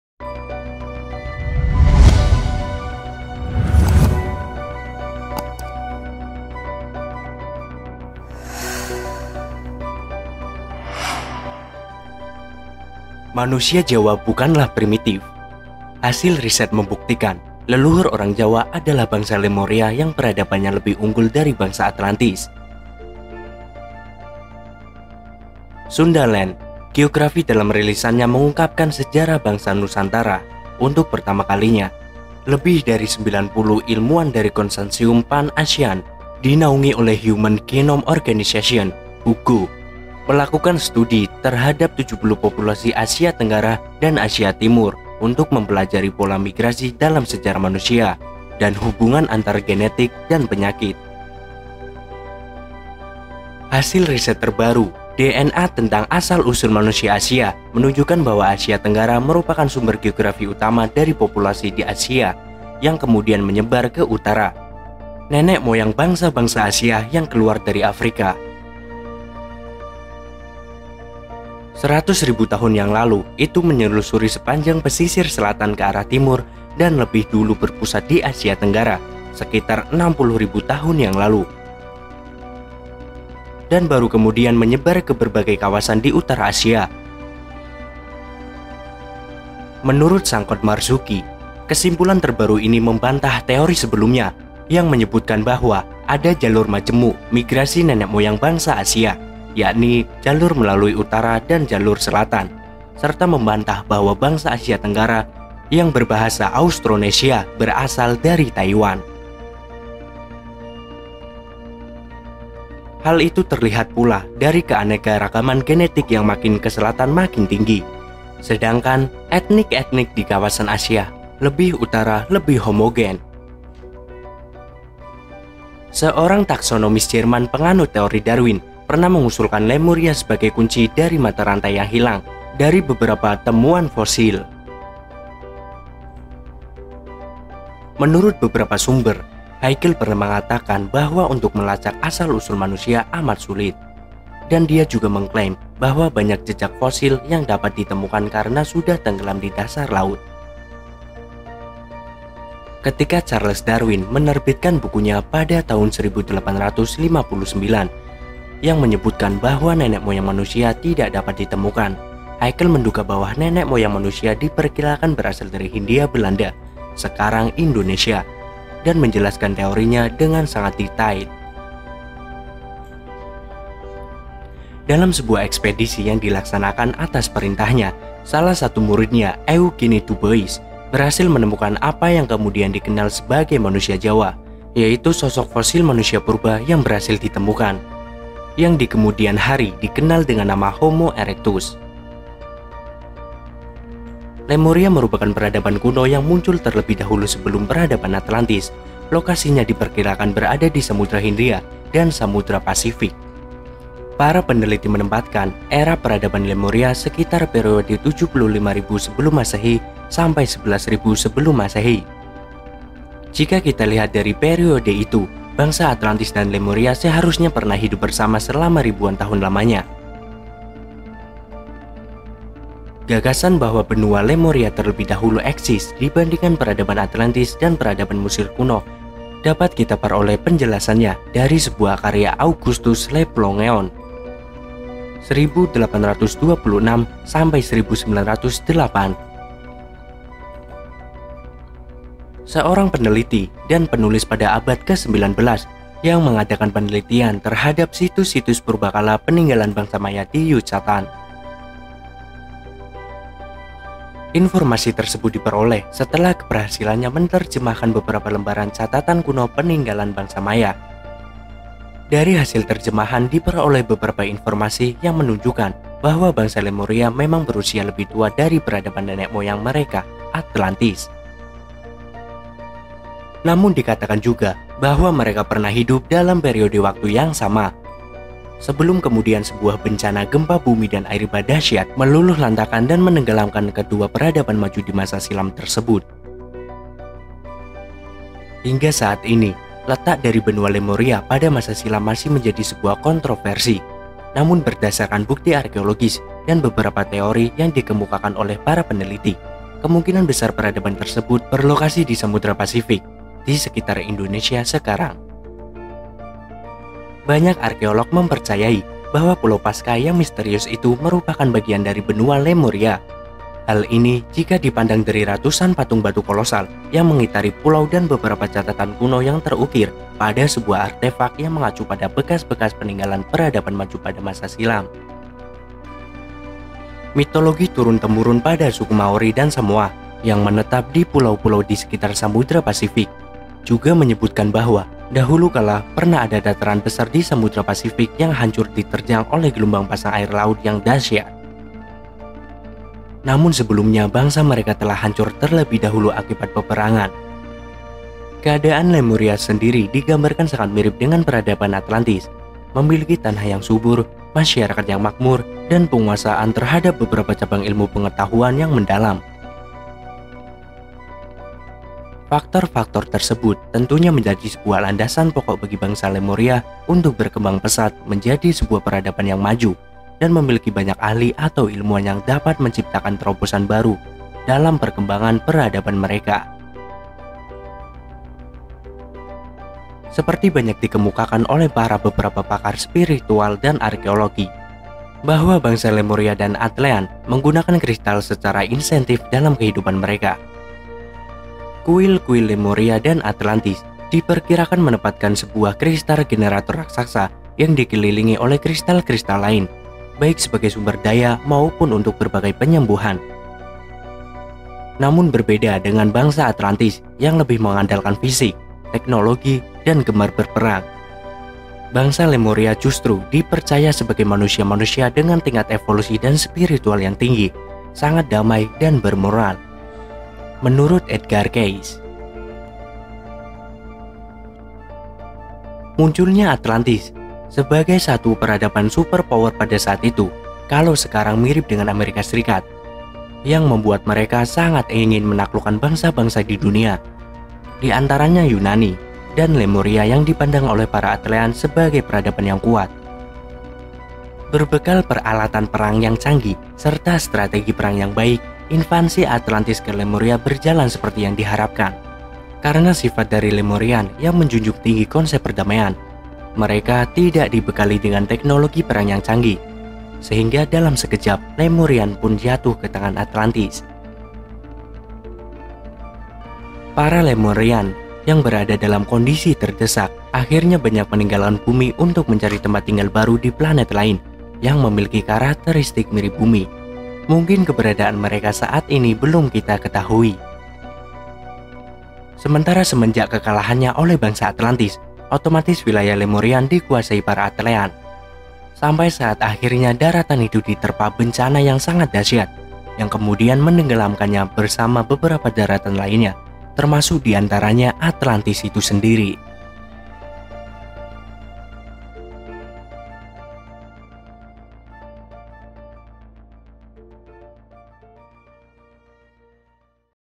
CERDAS...!!! PENELITI INI BERHASIL BUKTIKAN SUKU JAWA = BANGSA LEMURIA?!!
Sundaland Geografi dalam rilisannya mengungkapkan sejarah bangsa nusantara, Untuk pertama kalinya, lebih dari 90 ilmuwan dari konsorsium Pan-Asian SNP dinaungi oleh Human Genome Organisation (HUGO) melakukan studi terhadap 73 populasi Asia Tenggara dan Asia Timur untuk mempelajari pola migrasi dalam sejarah manusia dan hubungan antara genetik dan penyakit.